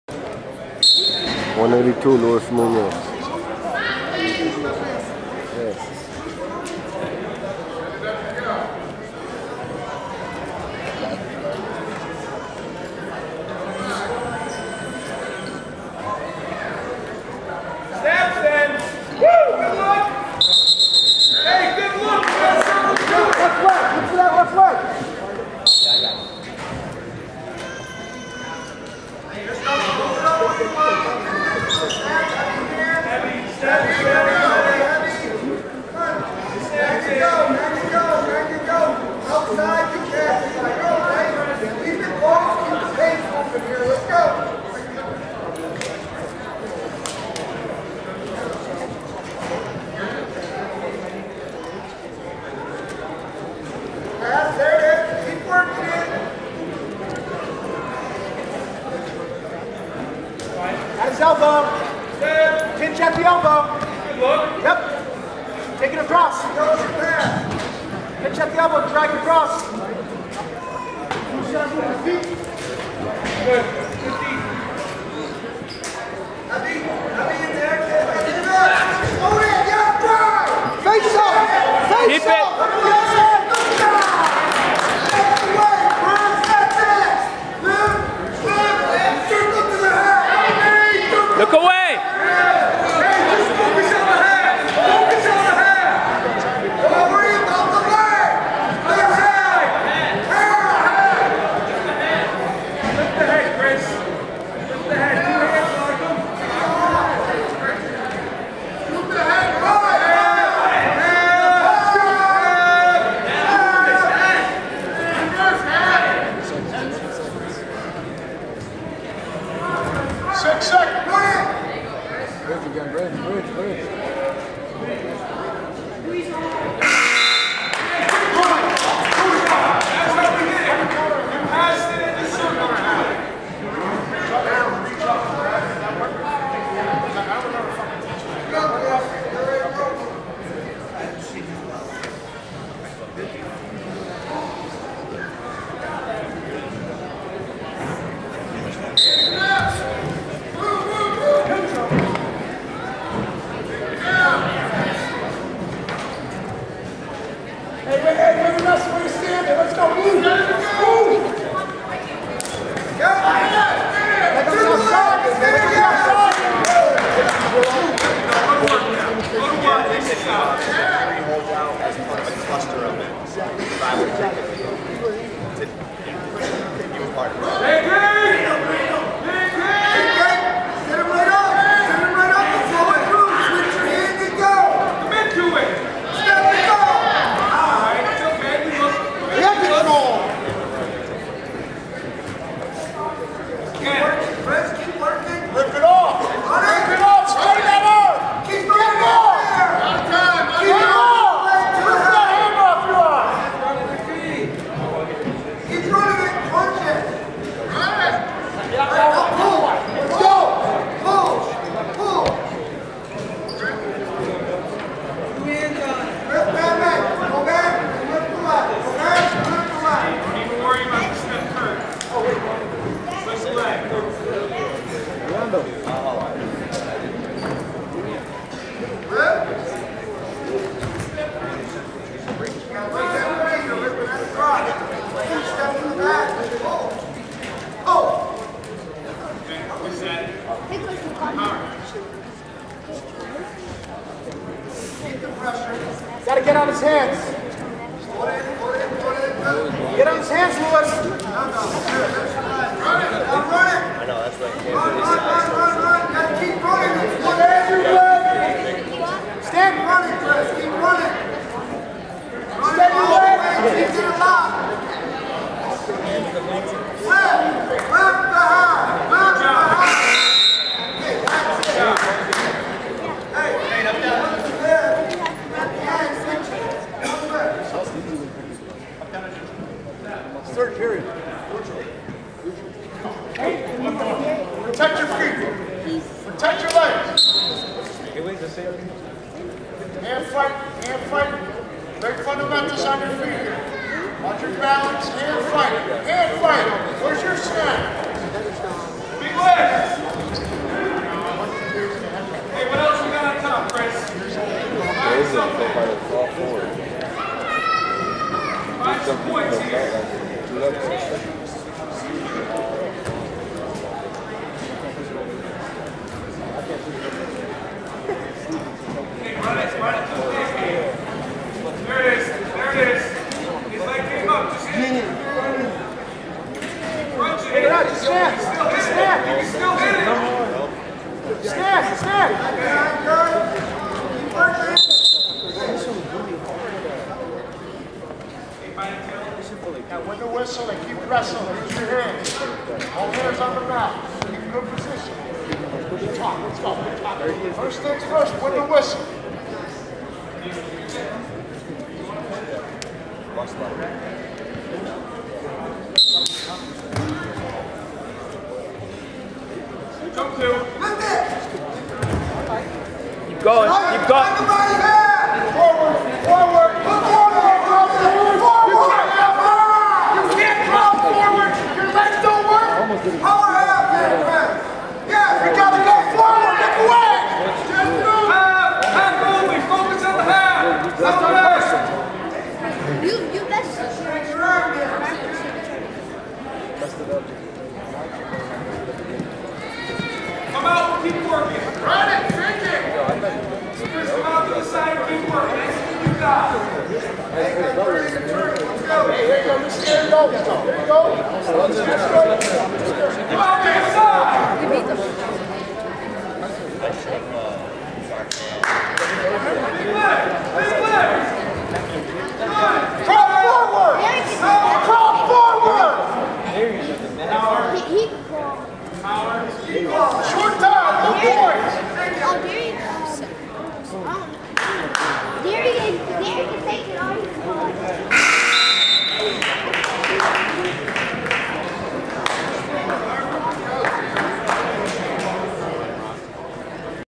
Luis Munoz (Central) vs. Chris Mayer (Mt. Anthony)
Luis Munoz (Central) vs. Chris Mayer (Mt. Anthony) in a 182 lb match of a duel meet between Central and  Mt Anthony at Central High School on December 20, 2017. Mayer wins 10-3. Mt. Anthony wins the match 57-6.